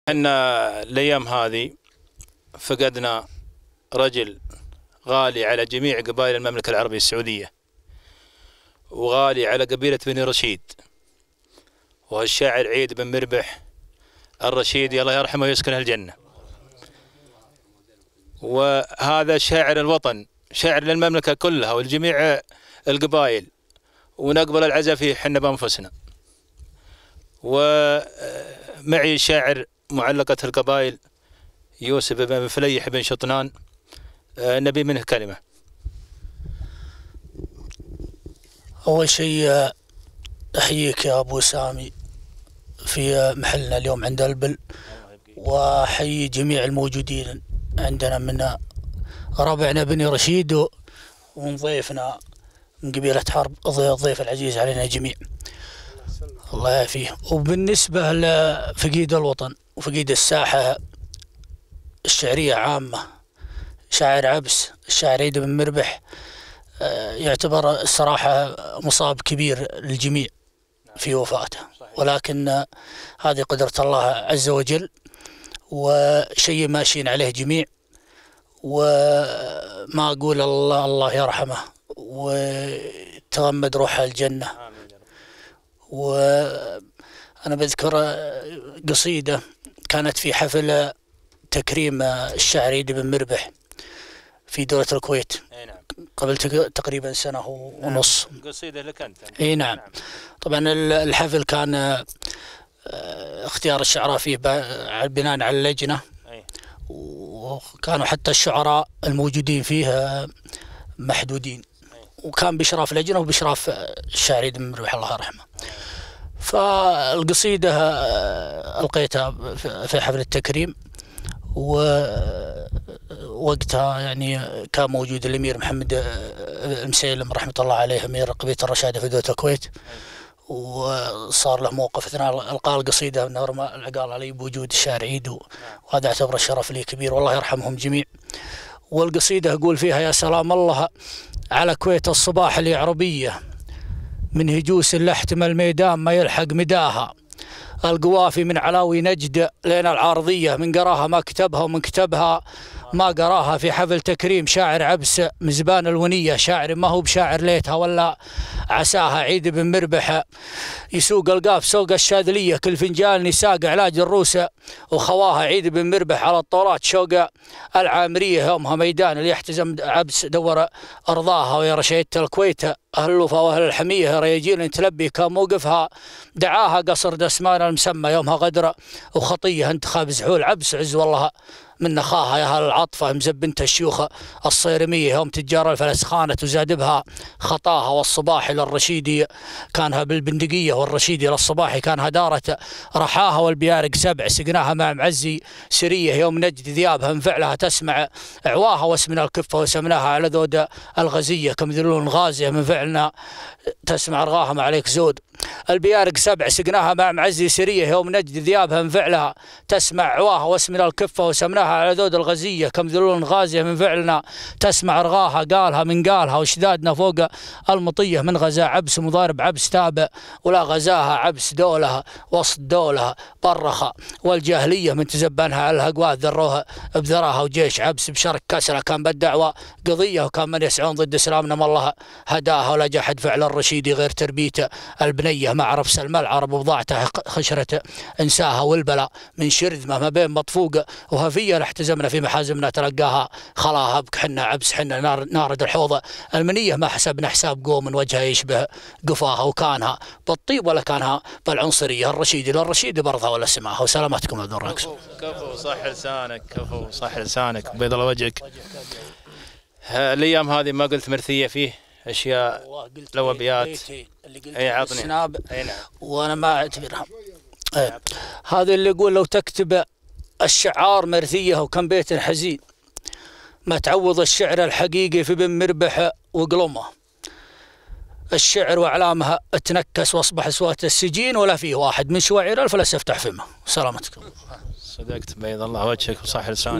الشاعر يوسف بن فليح بن شطنان الرشيدي في كلمه وقصيده عن الشاعر عيد بن مربح رحمه الله
الشاعر يوسف بن فلييح بن شطنان الرشيدي في كلمه وقصيده عن الشاعر عيد بن مربح رحمه الله